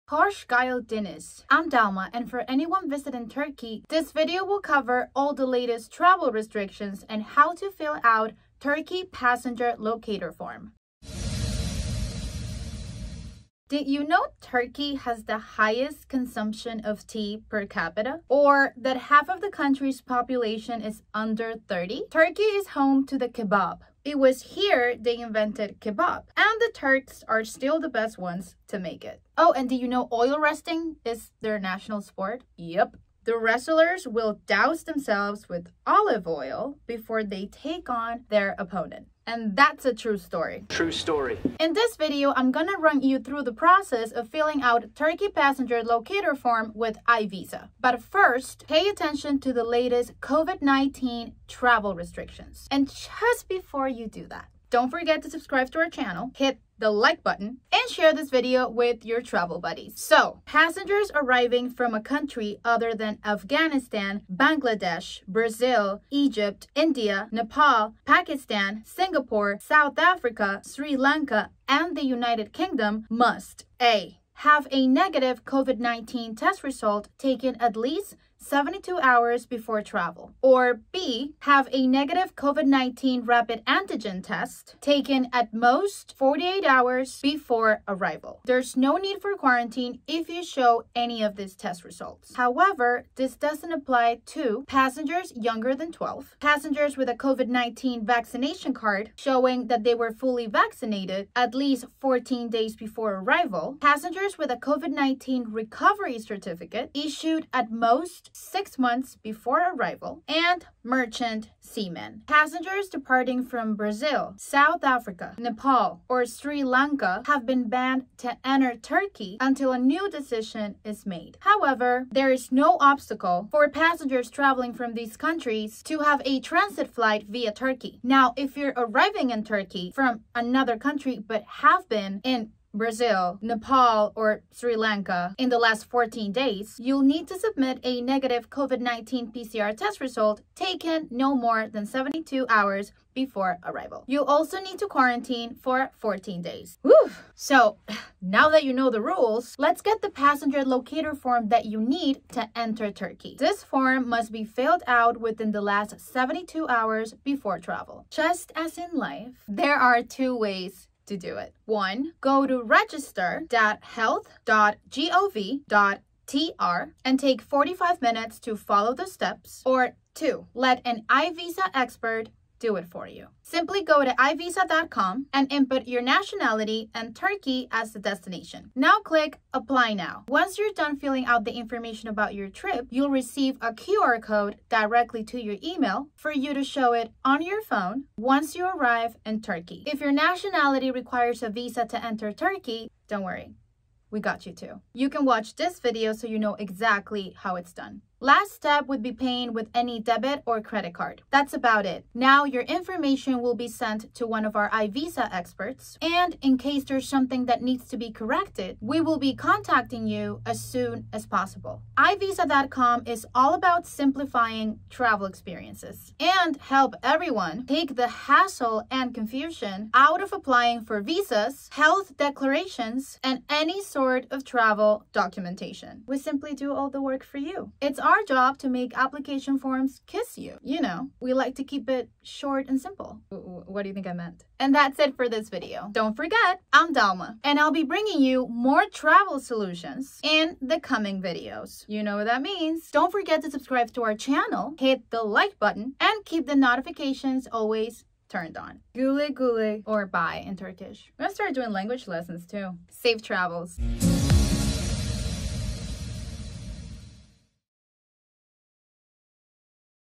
How to fill out Turkey Passenger Locator Form
Due to COVID-19, countries change travel regulations every day, so please visit our Learn More page for the most up-to-date information about traveling to Turkey

Did you know Turkey has the highest consumption of tea per capita? Or that half of the country’s population is under 30? In this video, we'll run you through the latest COVID-19 travel restrictions and how to get the Turkey Passenger Locator Form with iVisa.

00:00 Intro
00:47 Latest COVID travel restrictions per country and nationality
02:29 How to process and fill out the Turkey Passenger Locator Form
03:31 Why use iVisa to process your Passenger Locator Form